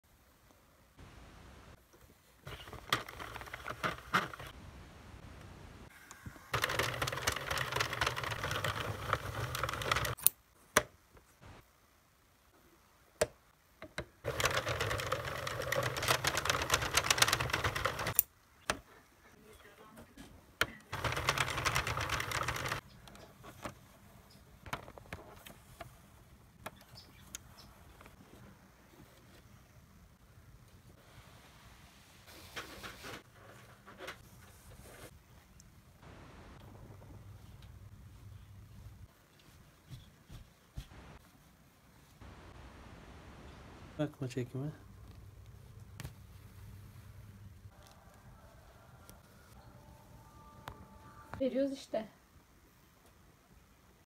Knitting Machine
Hello friends! In this video, I'm excited to share with you the amazing experience of knitting hats with a knitting machine. I meticulously recorded every detail using a special technique and expertly edited them together to create this fabulous knitting project. Enjoy watching!

Video Content:

Knitting hats with a knitting machine process
Detailed demonstration from start to finish
Different color options and patterns
Special touches on the hats
Don't miss out on this fun and inspiring knitting project! Wishing everyone happy knitting days!